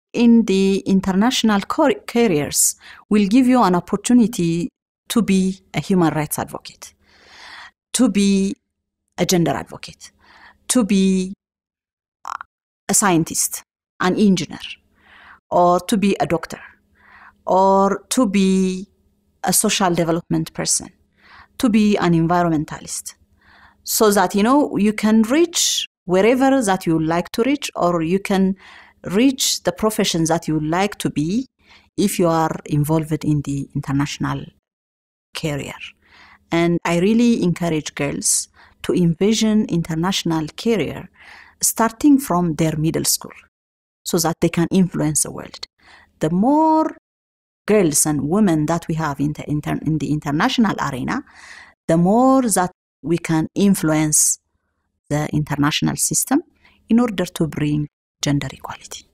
Why Consider International Development | Int. Development Specialist Meskerem Mulatu Legesse
Watch Meskerem Mulatu Legesse, a gender and energy consultant with The World Bank Group. She works to increase opportunities for women to pursue careers in STEM fields and leadership positions in Ethiopia's energy sector.